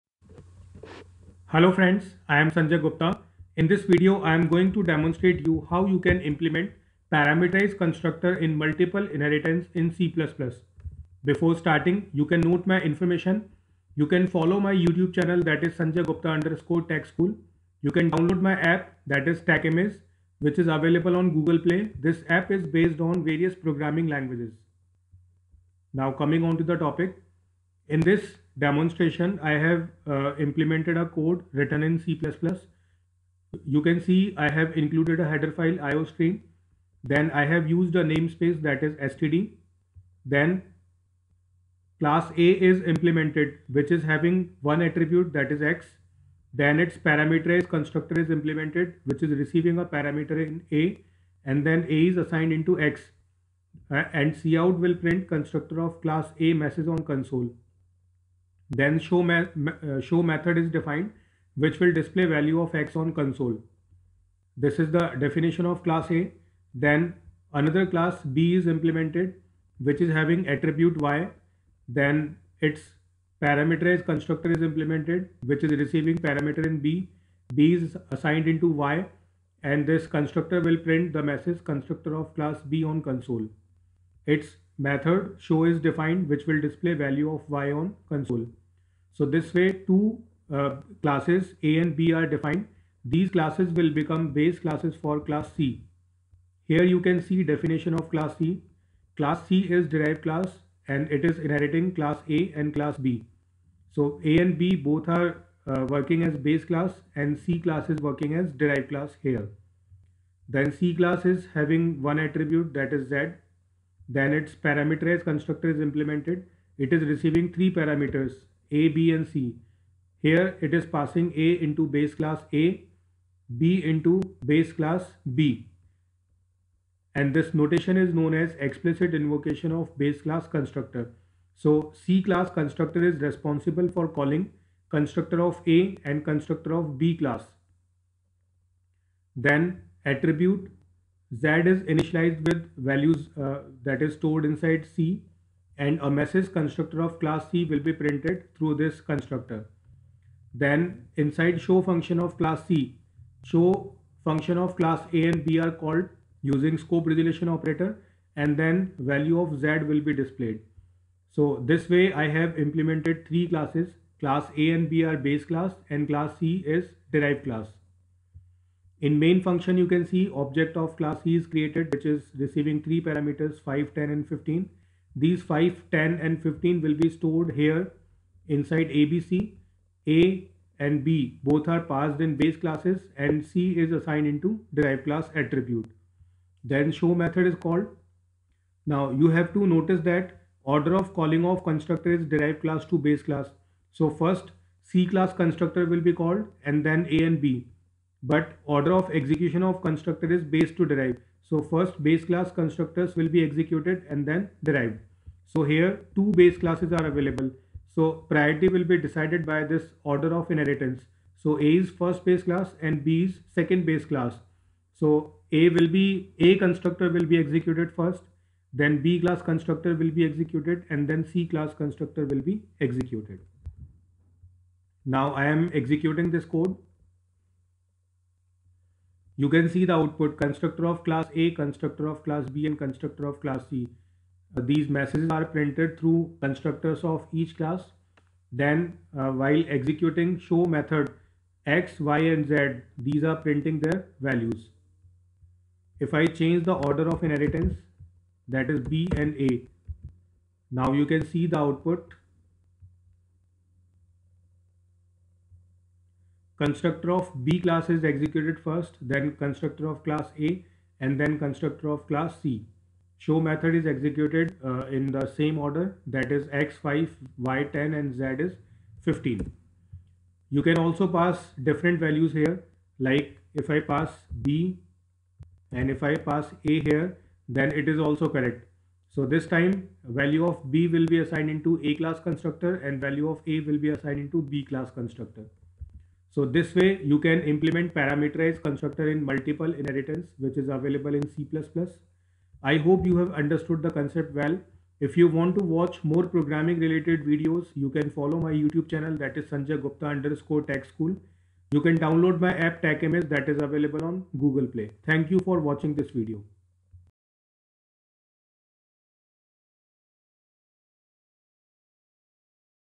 73 C++ | How to implement parameterized constructor in multiple inheritance in c++ | by Sanjay Gupta
Program to implement parameterized constructor in multiple inheritance in C++ programming language.